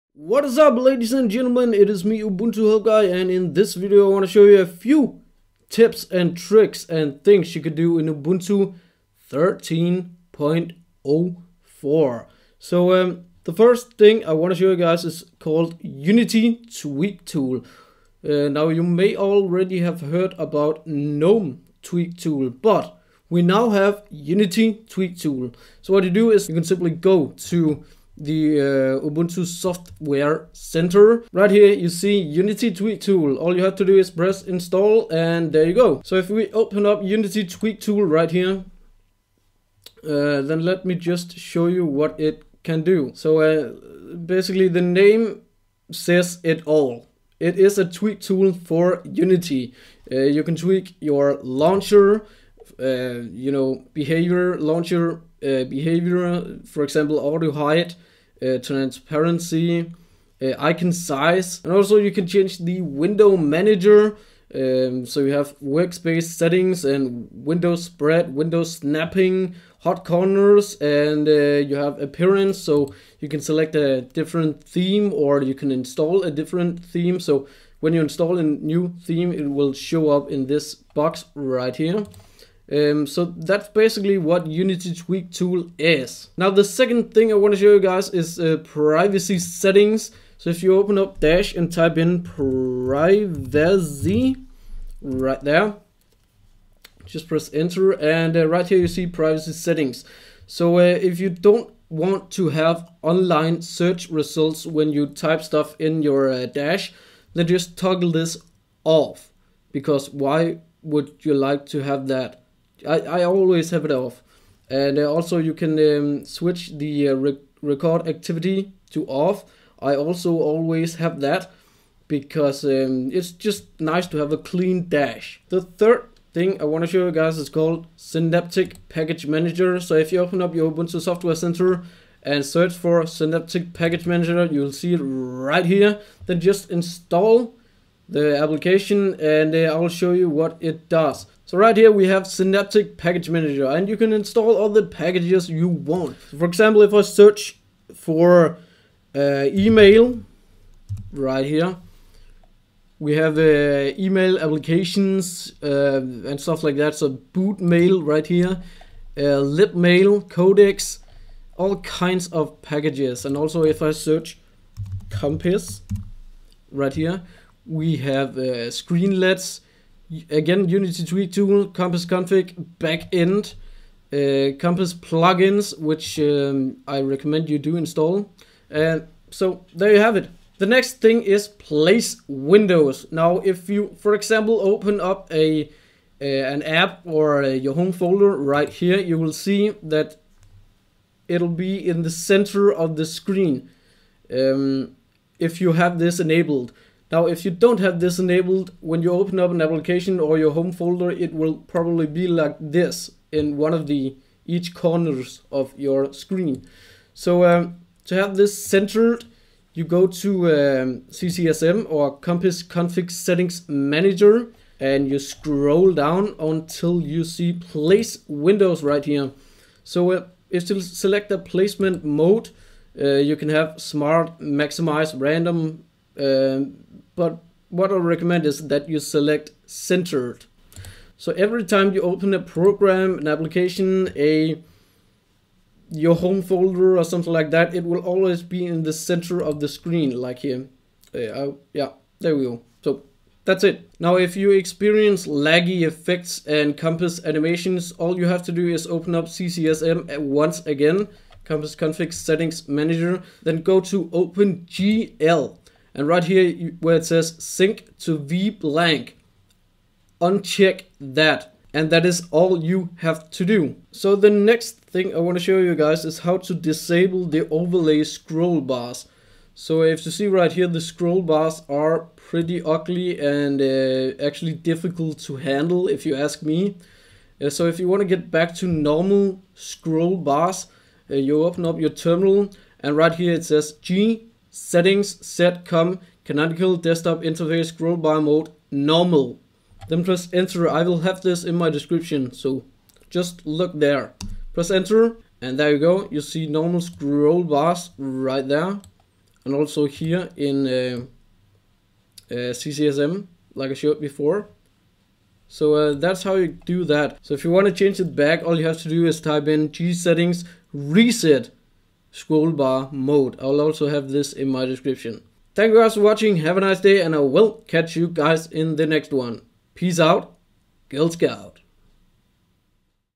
6 Useful Tips & Tricks - Ubuntu 13.04
In this video, I show you a few tips and tricks in Ubuntu 13.04. Remember to like the video and leave a comment on your thoughts!

I talk about these things in the video:
Unity Tweak Tool
Privacy Settings
Synaptic Package Manager
Place Windows
OpenGL

Disable overlay scrollbars:
gsettings set com.canonical.desktop.interface scrollbar-mode normal

Enable overlay scrollbars:
gsettings reset com.canonical.desktop.interface scrollbar-mode